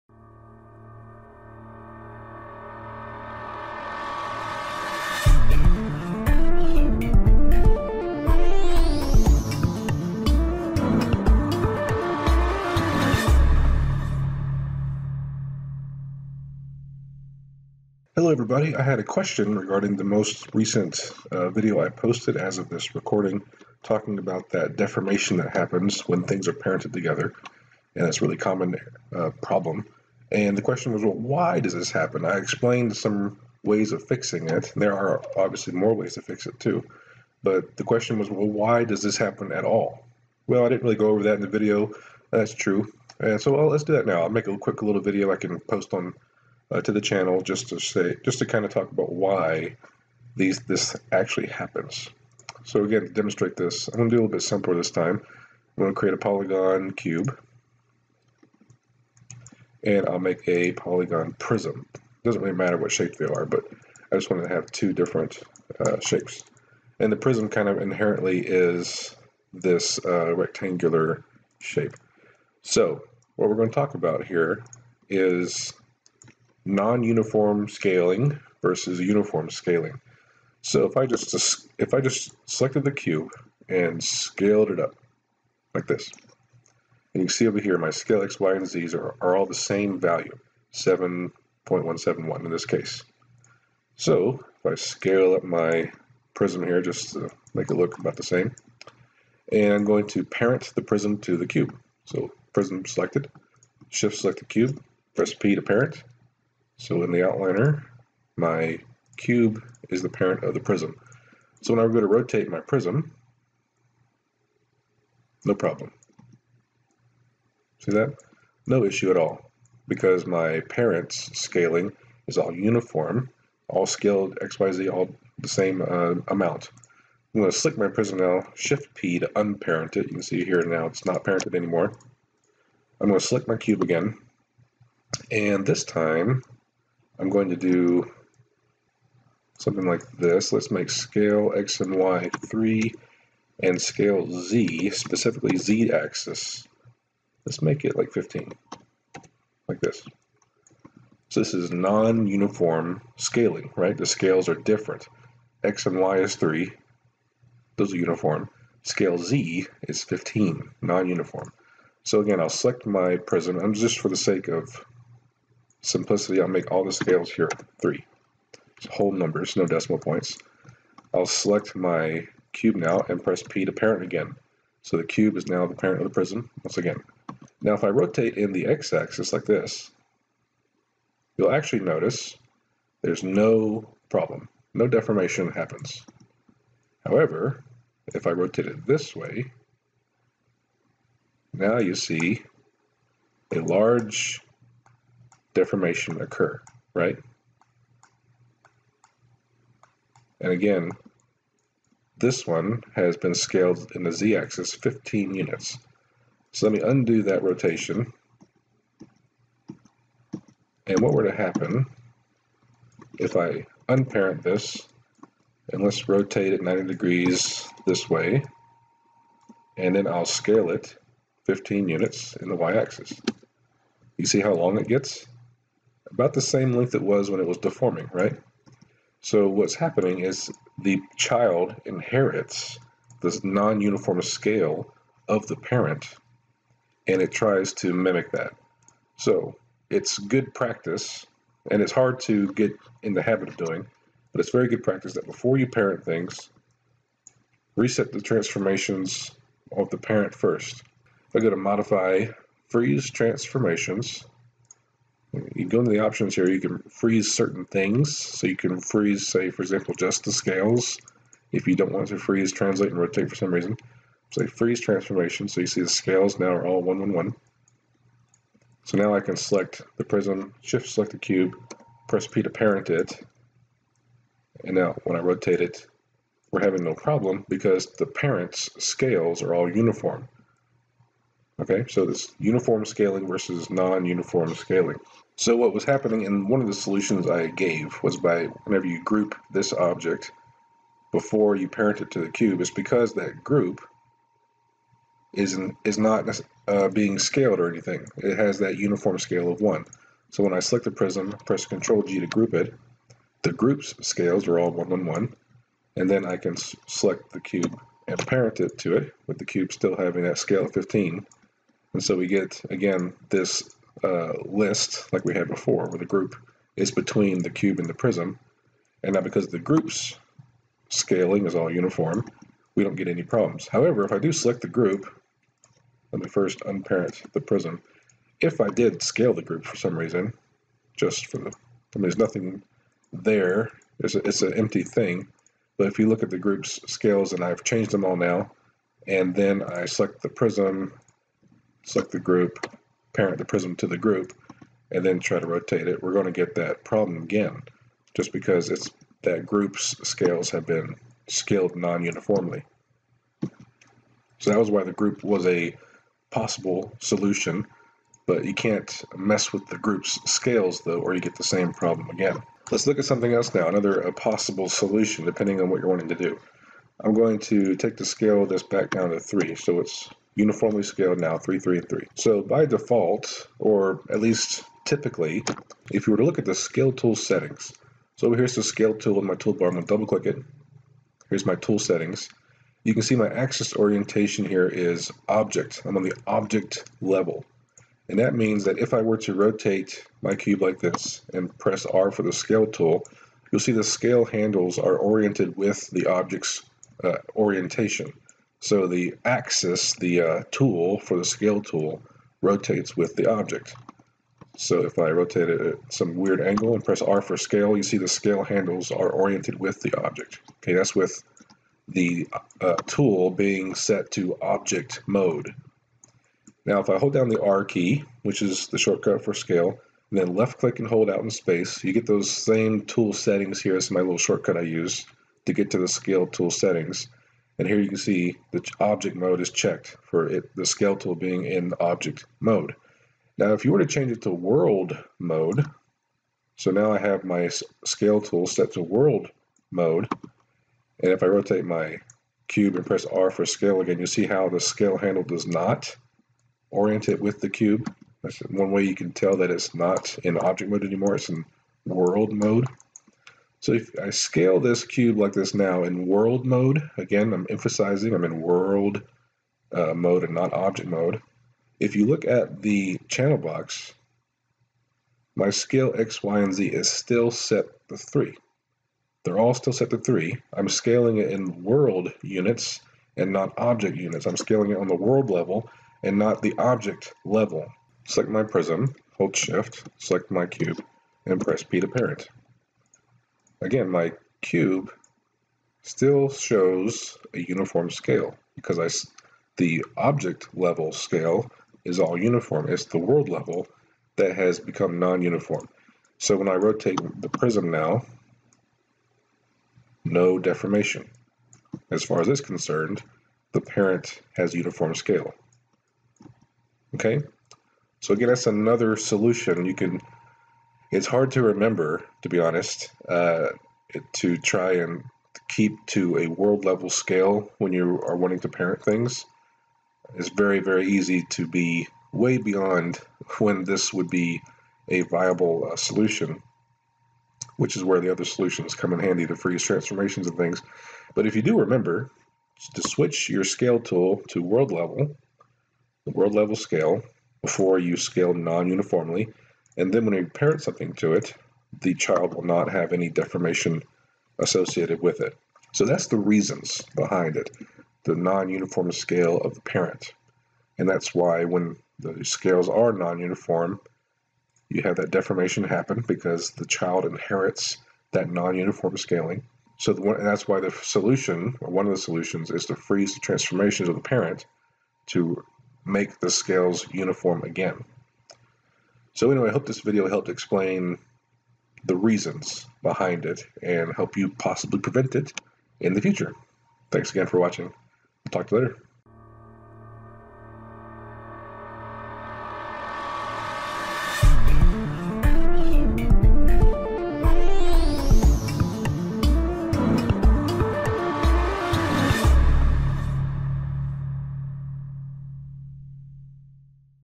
The Maya Toolbelt - Why Does Scale Deformation Happen?
Why does scale deformation happen when objects are parented or group? What is going on under the hood? We'll answer that here.

Product Links:
The Game Artist's Guide to Maya
The Game Animator's Guide to Maya
Studio Projects: Environments & Props